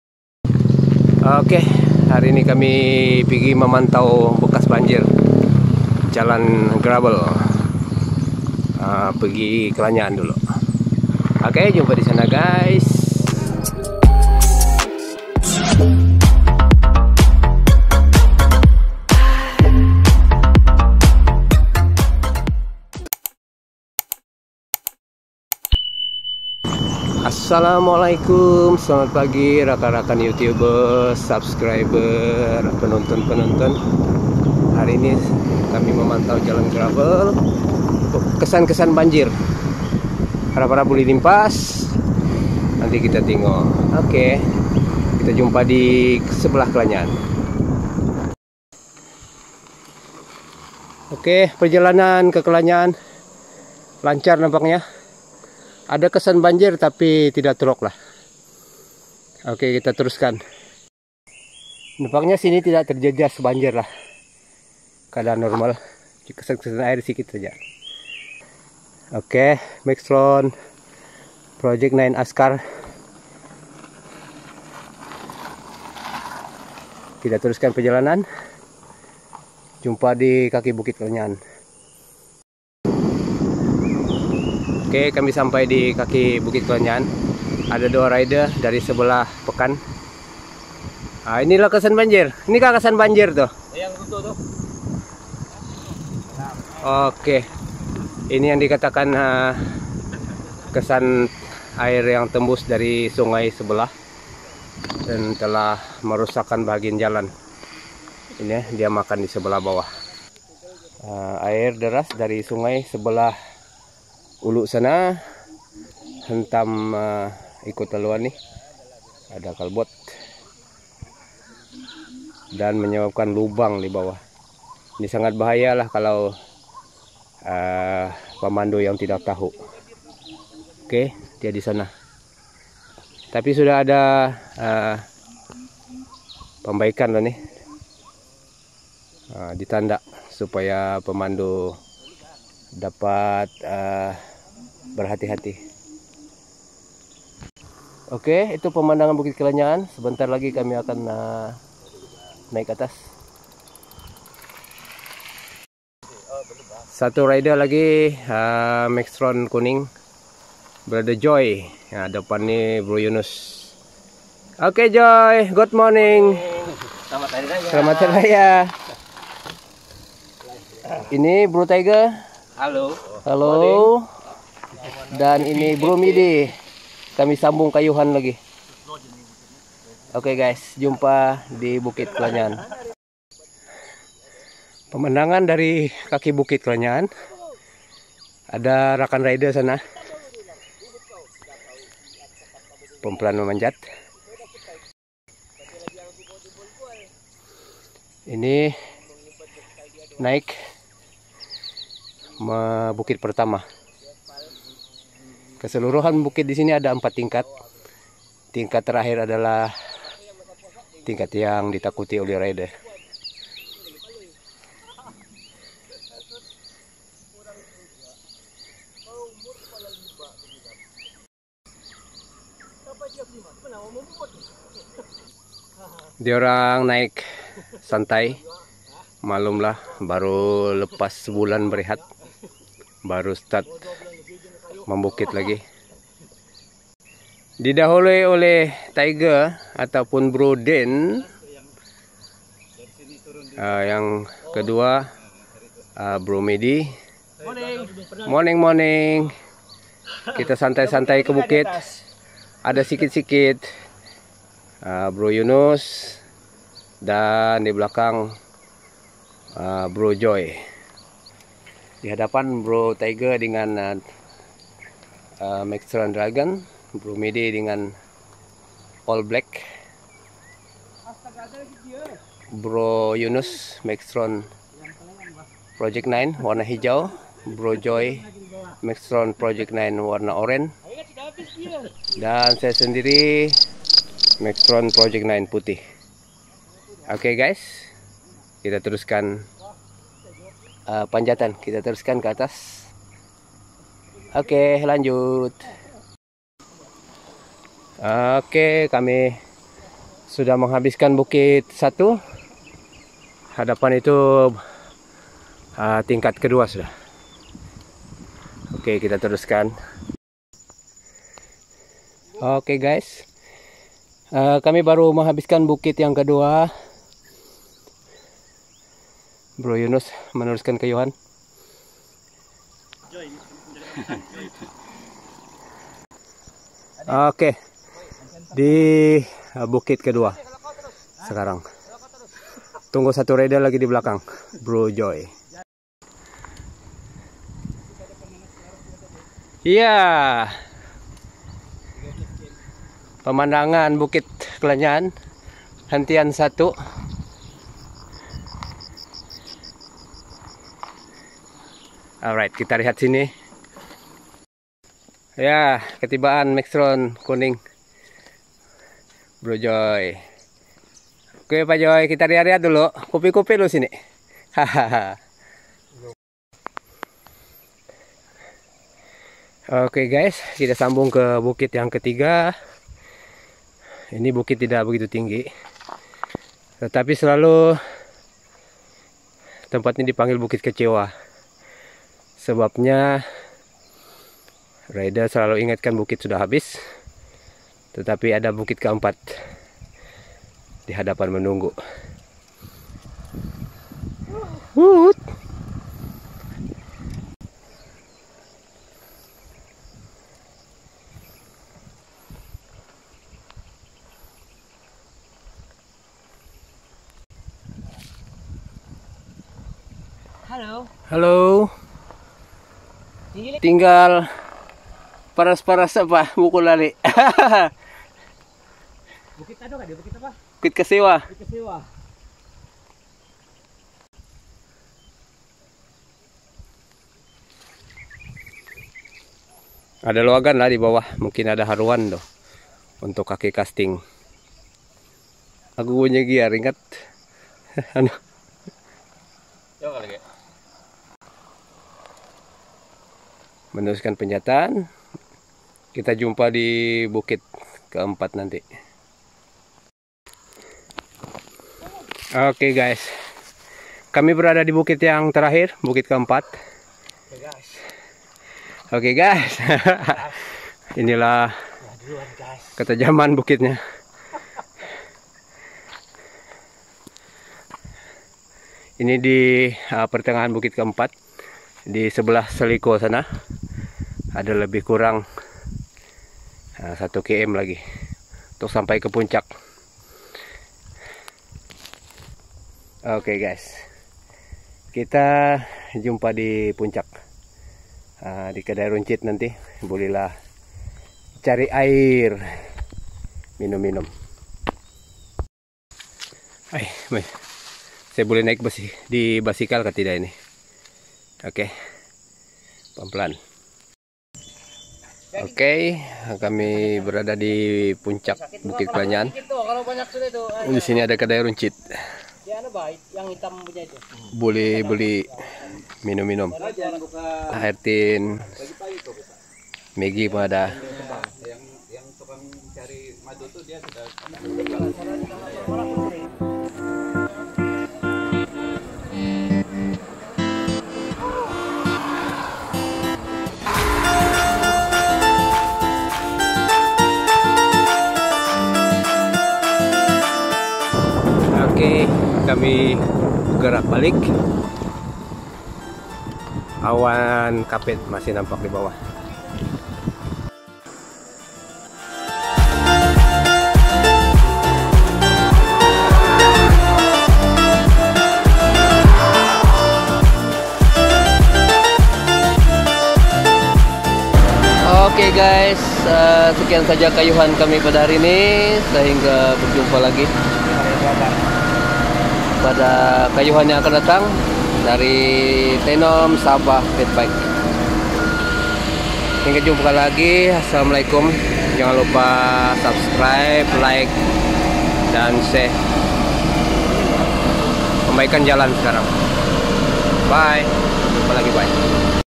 Kayuhan Selepas Banjir Laluan Kelanyaan Tenom Sabah
Kayuhan kali ini disertai 5 buah fat bike untuk melihat kesan banjir Tenom Sabah 2021. Selepas itu terus memanjat Bukit Kelanyaan untuk mengeluarkan peluh.